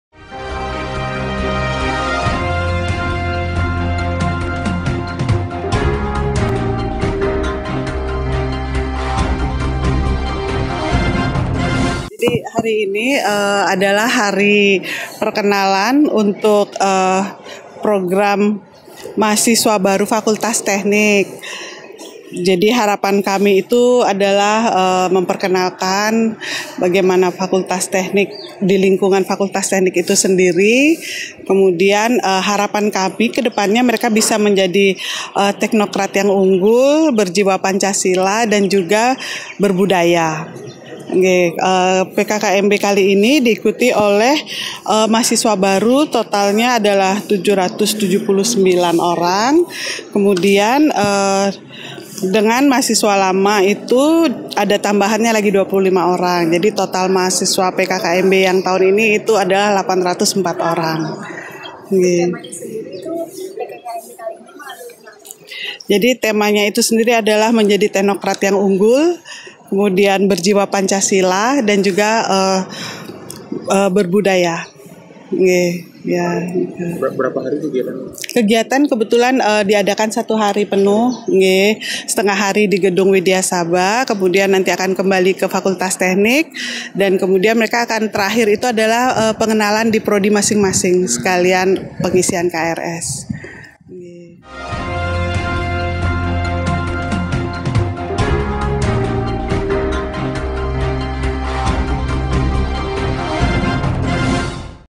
Wawancara Ketua Panitia PKKMB FT 2019
Fakultas Teknik Universitas Udayana melaksanakan Pengenalan Kehidupan Kampus Mahasiswa Baru (PKKMB) bertema “Menjadi Teknokrat yang Unggul, Berjiwa Pancasila dan Berbudaya”. Kegiatan dilaksanakan pada Jumat (16/8/2019) di Auditorium Wdyasabha, Kampus Bukit Jimbaran.
 Sebanyak 804 peserta tediri dari 779 orang mahasiswa baru dan 25 orang mahasiswa lama mengikuti PKKMB Fakultas Teknik. Dekan Fakultas Teknik, Prof. Ir. Ngakan Putu Gede Suardana M. T., Ph.D, berharap minimal 50% mahasiswa lulus tepat waktu. Pihaknya berharap agar kelulusan di Fakultas Teknik mampu mencapai 100%. Hal ini turut didorong melalui peningkatan kualifikasi dosen maupun peningkatan sarana prasarana.
 Harum Prabani, Febiano Ramadhan melaporkan untuk Udayana TV

Redaksi : Gedung GDLN Universitas Udayana Lantai 1
Telepon : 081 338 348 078